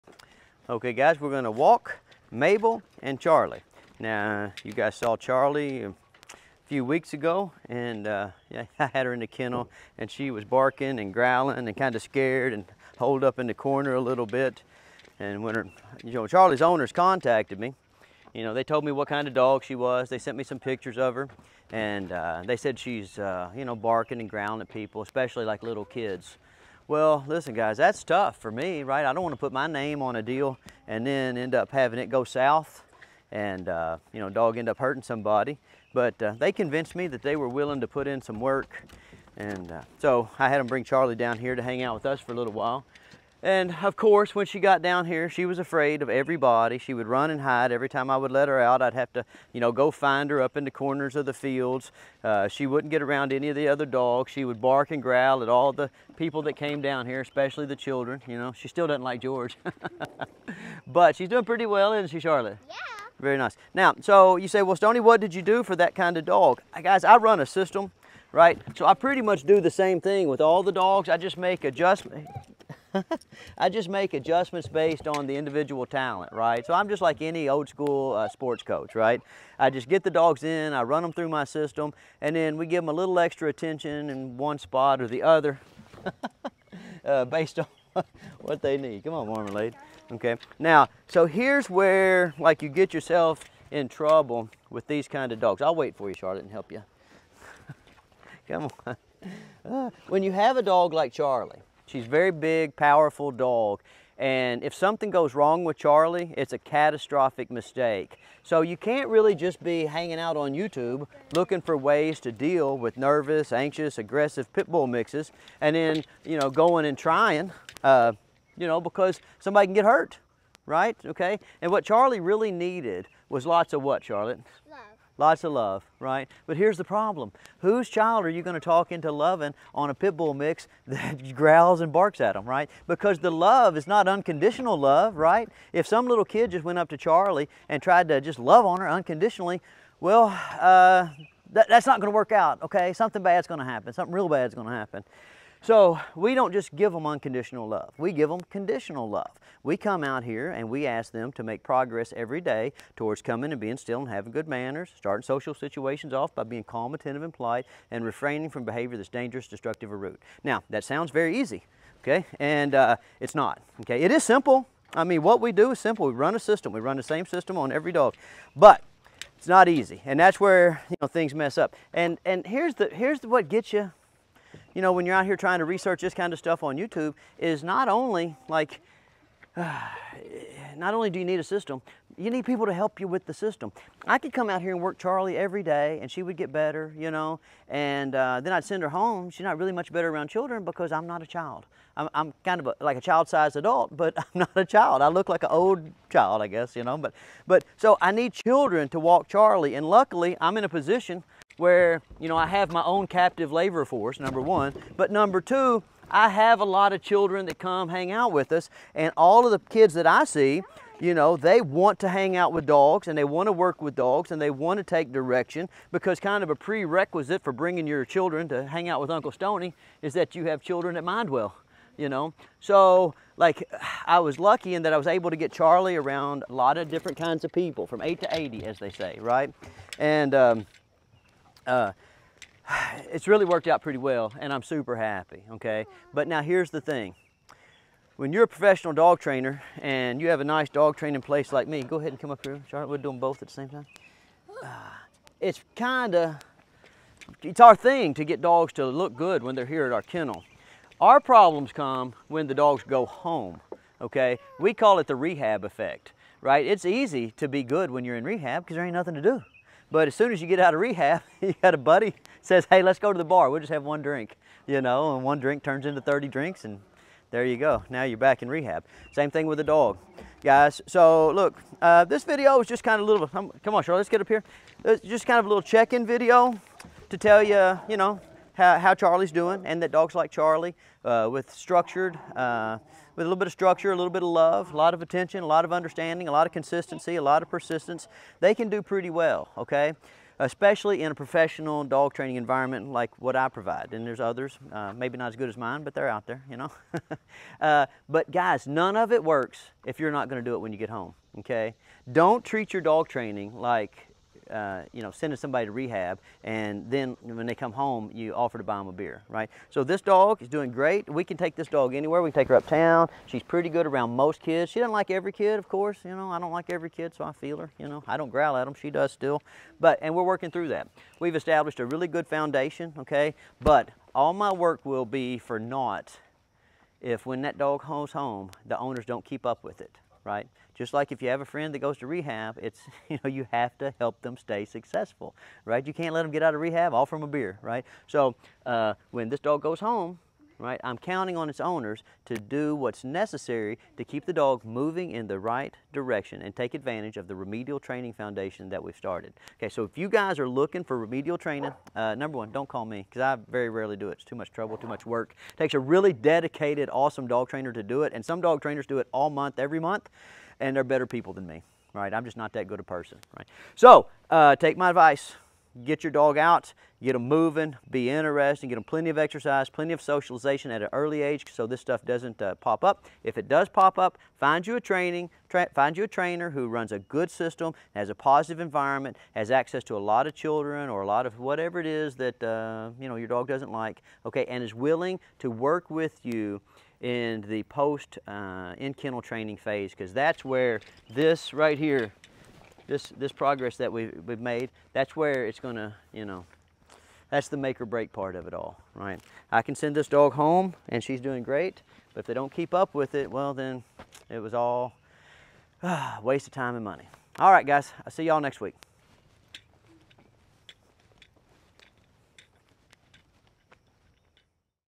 Aggressive/Fearful Dogs | The Keys To Dog Training Success
Today Uncle Stonnie gives an update on Charlie, an aggressive/fearful, Pit Bull mix that needed a little bit of remedial training and socialization.

In this video Stonnie provides a general outline of his thoughts concerning training aggressive/fearful dogs and their long term chances for successful reintegration into their home environments.

00:00 Introduction
01:00 We Run A System
01:40 Understanding Risk
02:26 Conditional Love
03:01 Proper Training Takes A Village
04:11 The Rehab Effect
05:24 Why Professional Training Fails

We hope you guys enjoy!